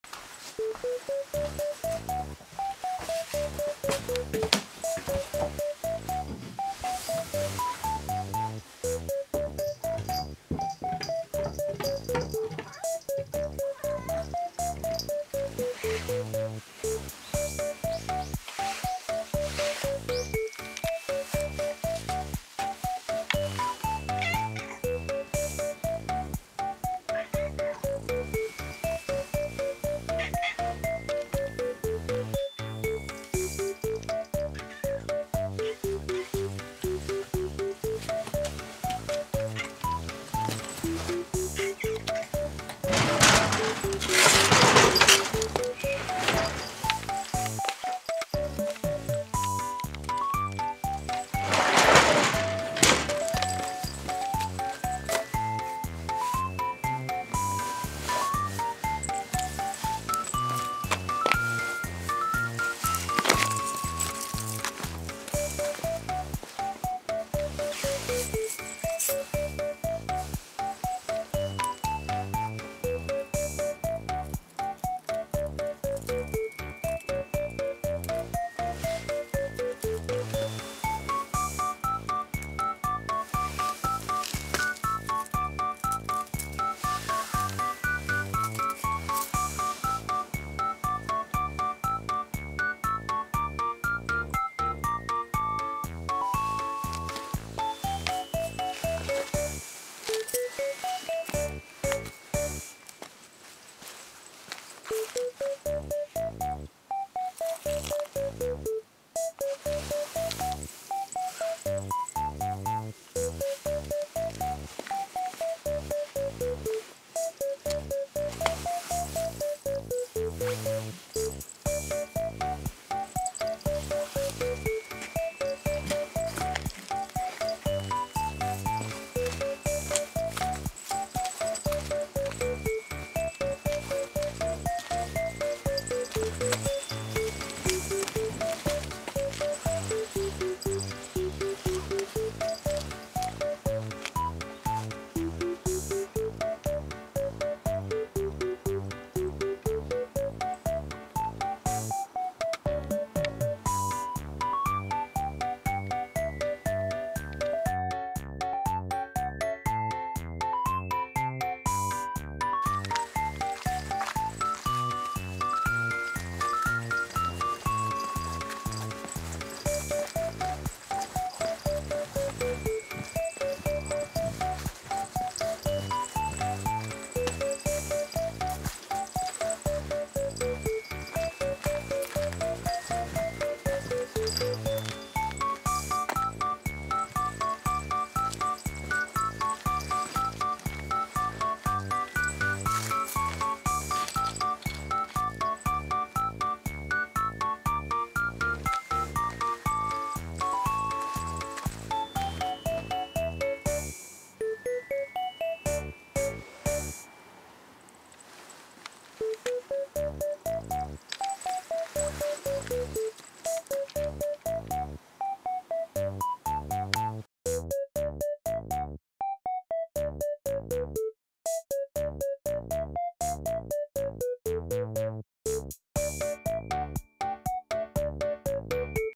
今日も外出るぞ。ちょっと趣向変えて・・【侵入した野良仔猫】～家猫修行中～
外へ出るといつも同じところばかり・・
これでは世間知らずの井の中の蛙のお山の大将になってしまう！
なのでちょっと冒険（？）してみました・・